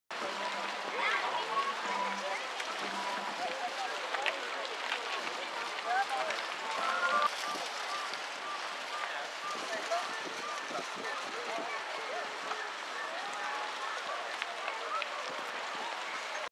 Kites Dancing in the Wind Marseille
Marseille Kite festival - See 50+ videos like this This is just one of over 50 videos from the Marseille Kite festival - Fete du Vent - A Gust of Art (Festival of Wind) held at Parc Balneaire du Prado in Marseille on the 13th to 15th September 2013, this footage was recorded on the 13th. The sky was quite literally filled with kites of all shapes, sizes and colours flying around in the wonderful blue skies over the park and beach (location It was the 28th edition of the festival and an amazing performance featuring stands and delegations from all corners of the world. You will see quite a few kites featured by Peter Lynn Some of the kites to be seen include a Sea Horse, Octopus, Ray and Jellyfish to name but a few.